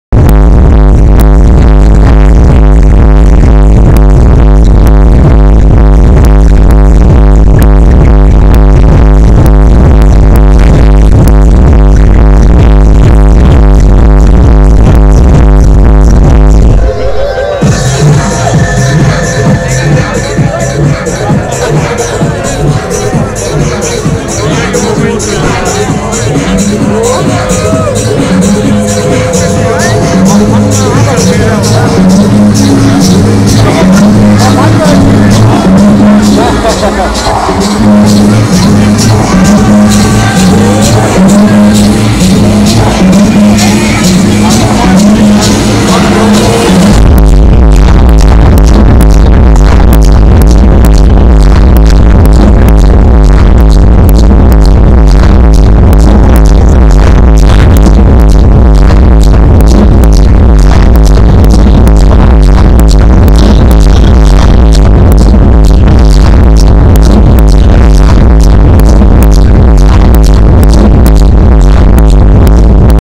Distortion 2012 - Final Party - Refshaleøen - Rave
The Amazing Everyday præsenterer Distortion Daily Documentaries. 8 Distortion deltagere vil dokumentere deres oplevelse af festivalen med deres Nokia Lumia 900 og hylde den største københavnerfest. Se videoerne dag for dag, bydel for bydel, på